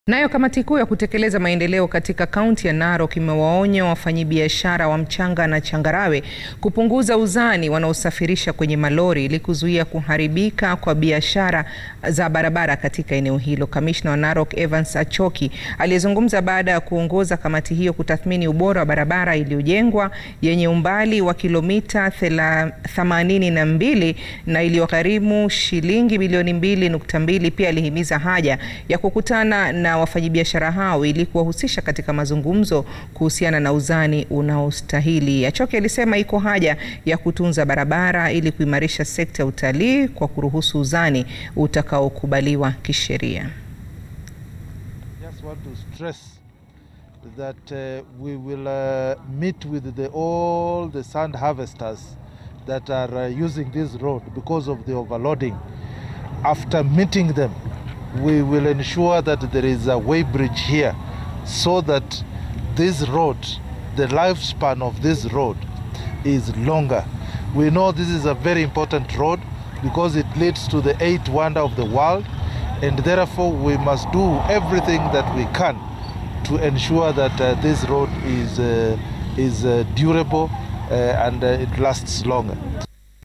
Wachimbaji changarawe kaunti ya Narok waonywa kuhusu uzani wanaosafirisha
Kamati kuu ya kutekeleza Maendeleo katika Kaunti ya Narok imewaonya wanaovuna changarawe kupunguza uzani wanaosafirisha kwenye malori ili kuzuia kuharibiwa kwa barabara za eneo hilo.